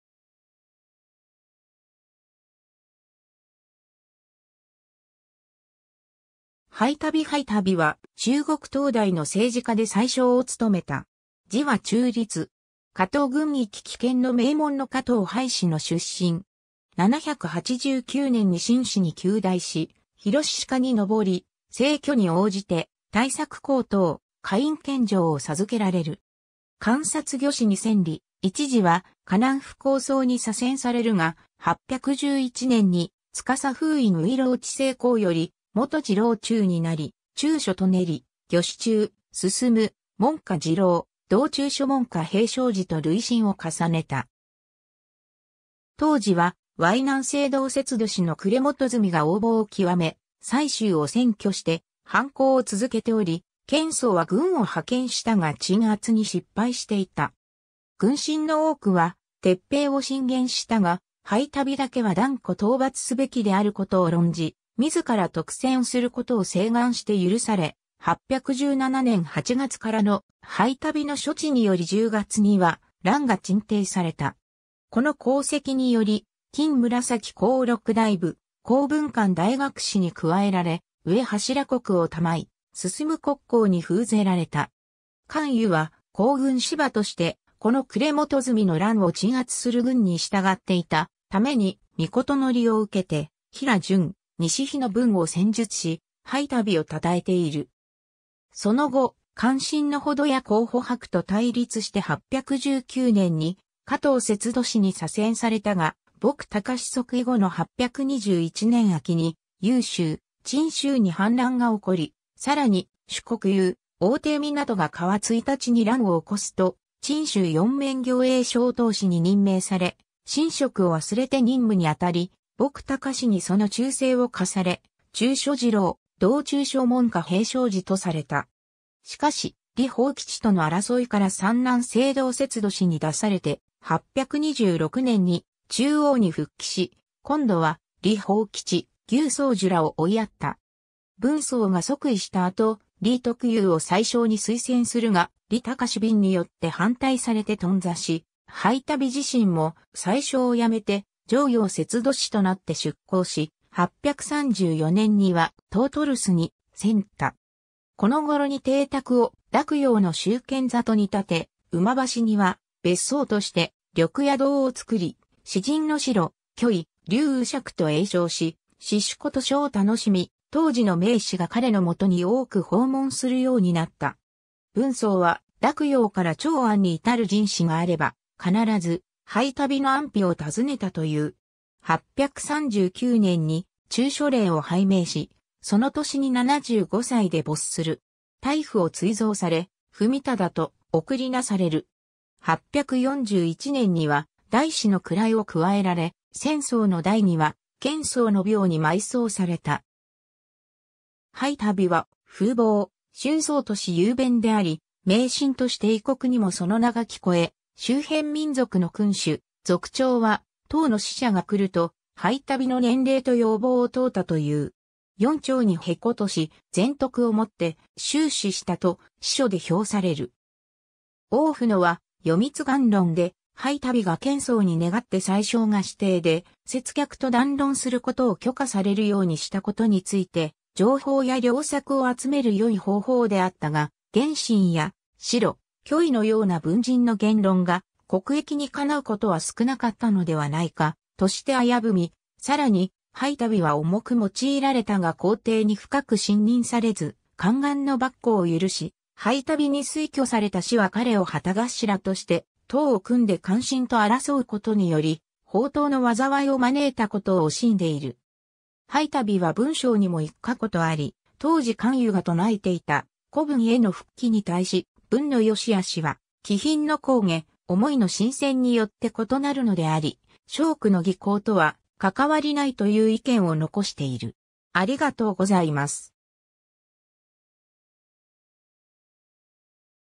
裴度
裴度 裴 度（はい ど、765年 - 839年）は、中国唐代の政治家で宰相を勤めた。
字は中立。
 河東郡聞喜県（現在の山西省運城市聞喜県）の名門の河東裴氏の出身。
789年に進士に及第し宏詞科に登り、制挙に応じて対策高等・河陰県尉を授けられる。
監察御史に遷り、一時は河南府功曹に左遷されるが、811年に司封員外郎知制誥より本司郎中になり、中書舎人・御史中丞・門下侍郎・同中書門下平章事と累進を重ねた。
当時は淮南西道節度使の呉元済が横暴を極め蔡州を占拠して反抗を続けており、憲宗は軍を派遣したが鎮圧に失敗していた。
群臣の多くは撤兵を進言したが、裴度だけは断固討伐すべきであることを論じ、自ら督戦することを請願して許され、817年8月からの裴度の処置により10月には乱が鎮定された。
この功績により金紫光禄大夫・弘文館大学士に加えられ、上柱国を賜い晋国公に封ぜられた。
韓愈は行軍司馬として、この呉元済の乱を鎮圧する軍に従っていたために、詔を受けて「平淮西碑」の文を撰述し、裴度を称えている。
 その後、姦臣の程异や皇甫鎛と対立して819年に河東節度使に左遷されたが、穆宗即位後の821年秋に幽州・鎮州に反乱が起こり、さらに朱克融・王廷湊が河朔に乱を起こすと、鎮州四面行営招討使に任命され寝食を忘れて任務にあたり、穆宗にその忠誠を嘉され中書侍郎・同中書門下平章事とされた。
しかし、李逢吉との争いから山南西道節度使に出されて、826年に中央に復帰し、今度は李逢吉・牛僧孺らを追いやった。
文宗が即位した後、李徳裕を宰相に推薦するが、李宗閔によって反対されて頓挫し、裴度自身も宰相を辞めて襄陽節度使となって出向し、834年には東都（洛陽）留守に遷った。
 この頃に邸宅を洛陽の集賢里に建て、午橋には別荘として緑野堂を造り、詩人の白居易・劉禹錫と詠唱し詩酒琴書を楽しみ、当時の名士が彼のもとに多く訪問するようになった。
文宗は洛陽から長安に至る人士があれば必ず裴度の安否を尋ねたという。
839年に中書令を拝命し、その年に75歳で没する。
太傅を追贈され、文忠と諡される。
841年には太師の位を加えられ、宣宗の代には憲宗の廟に埋葬された。
 裴度は風貌俊爽とし雄弁であり名臣として異国にもその名が聞こえ、周辺民族の君主・族長は唐の使者が来ると裴度の年齢と容貌を問うたという。
「四朝に歴事し、全徳を以て終始した」と史書で評される。
 王夫之は『読通鑑論』で、裴度が憲宗に願って宰相が私邸で説客と談論することを許可されるようにしたことについて、情報や良策を集める善い方法であったが元稹や白居易のような文人の言論が国益にかなうことは少なかったのではないか、として危ぶみ、さらに裴度は重く用いられたが皇帝に深く信任されず宦官の跋扈を許し、裴度に推挙された士は彼を旗頭として党を組んで姦臣と争うことにより、朋党の禍を招いたことを惜しんでいる。
 裴度は文章にも一家言あり、当時韓愈が唱えていた「古文」への復帰に対し、「文の善し悪しは気品の高下、思いの深浅によって異なるのであり、章句の技巧とは関わりない」という意見を残している（『寄李翺書』）。